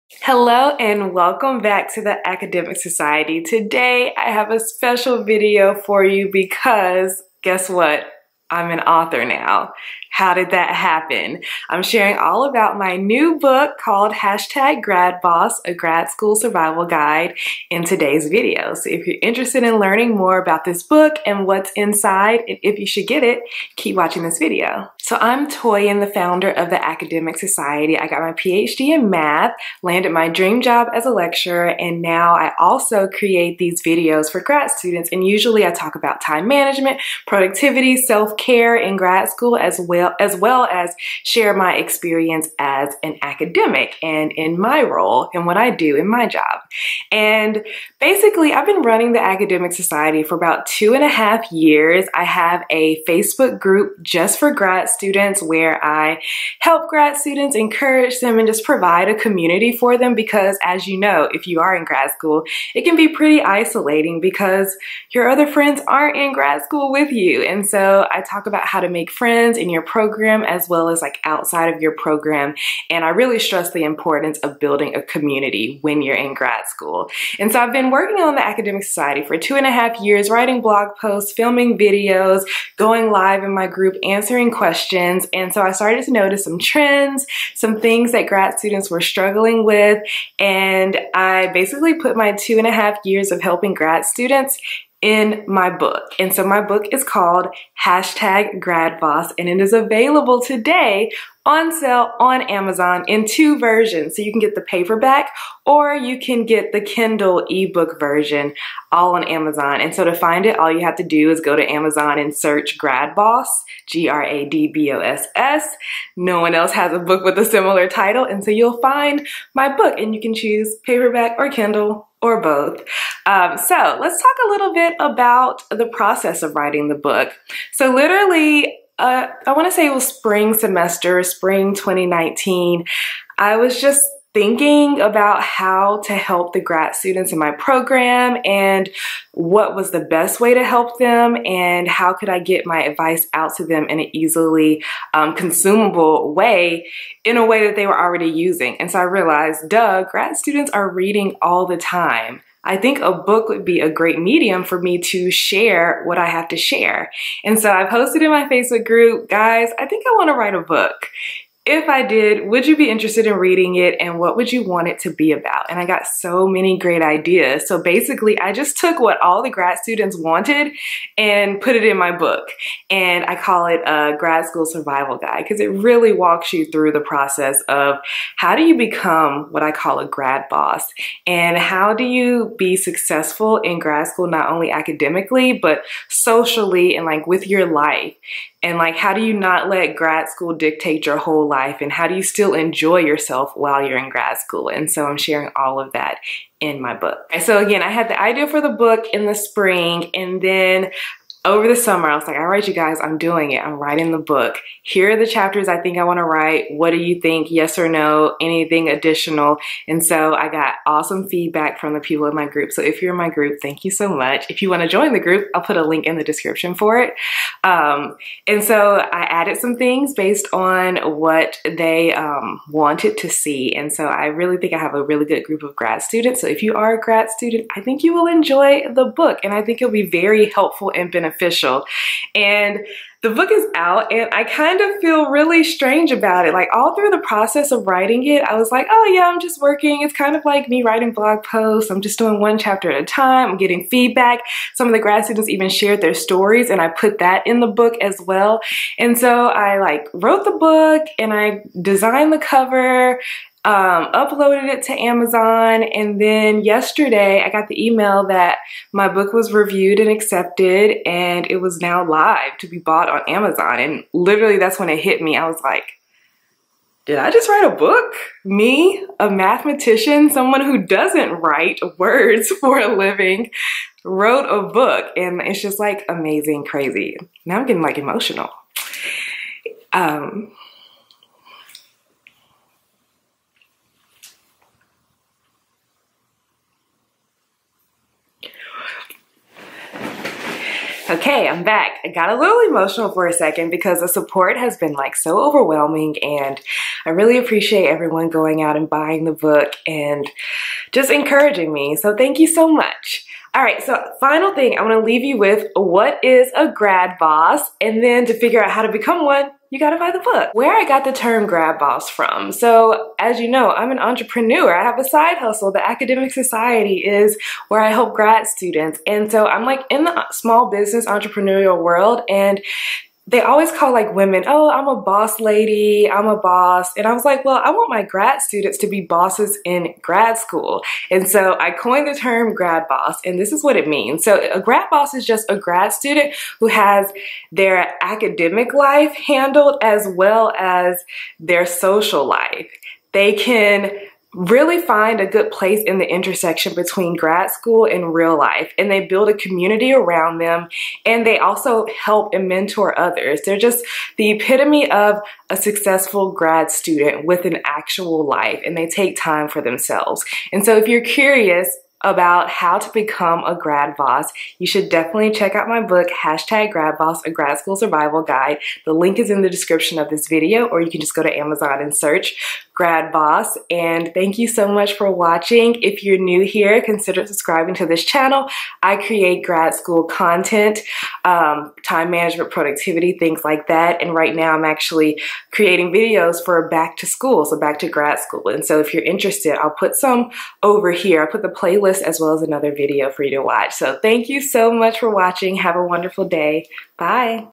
How to Become a GRADBOSS | A Grad School Survival Guide
Omg you guys! I did it! I wrote a book! It feels surreal! Like it’s just hitting me that I really wrote a book. Now that it’s live!

Yes! My book GRADBOSS: A Grad School Survival Guide is Live now on Amazon to be purchased on paperback or Kindle!

If you go to Amazon and search “gradboss”, you will find it.

Here are the deets:

GRADBOSS is the ultimate grad school survival guide. GRADBOSS includes worksheets, templates, workflows, and actionable advice from a millennial who got her PhD in Math and landed her dream job before graduation.

A gradboss is a grad student who has figured out how to balance grad school and real life. They are productive but they also have a social life. In addition to killing it academically, they've built a supportive community around them AND they help others. Can you imagine being completely successful in grad school without being overwhelmed?

GRADBOSS walks you through
-preparing for a new semester of grad school
-creating a productive weekly schedule
-setting realistic goals
-being productive despite having unstructured time
-cultivating meaningful relationships
-choosing an advisor
-handling failure in grad school
-having a life outside of grad school

Are you ready to be a gradboss? Don’t forget to share this with your grad school friends!